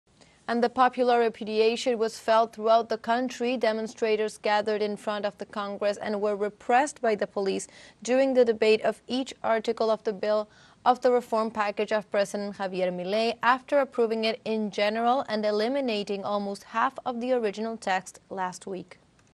Argentina: police repressed demonstrators crowded in front of Congress
Argentine police repressed protesters who gathered in front of Congress demanding their rejection of the controversial Omnibus Law. teleSUR